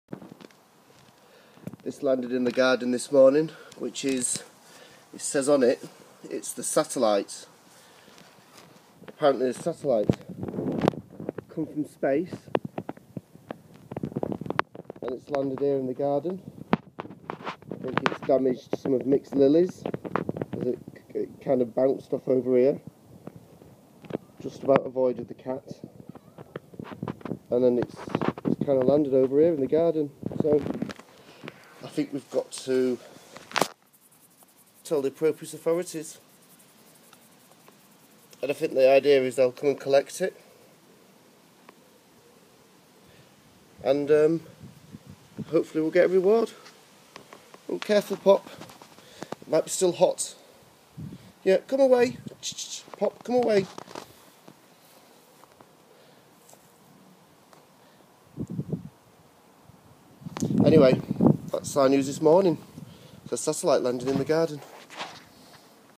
NASA Satellite Parts Crashed in our Garden!
NASA satellite falling to earth landed here in our garden. It damaged Mick's lilies and scared Pop.